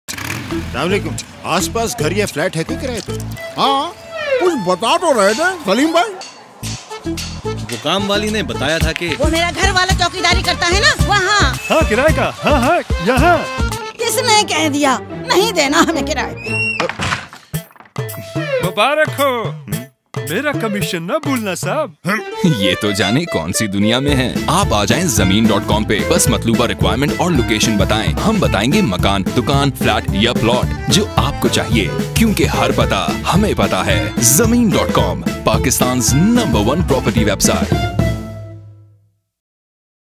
"Har Pata, Humain Pata Hai" - Zameen.com's Television Commercial
Tired of knocking on the wrong doors and having them slammed in your face? Log on to Zameen.com, Pakistan's No.1 property website, and find your dream home, kyun ke Har Pata, Humain Pata Hai!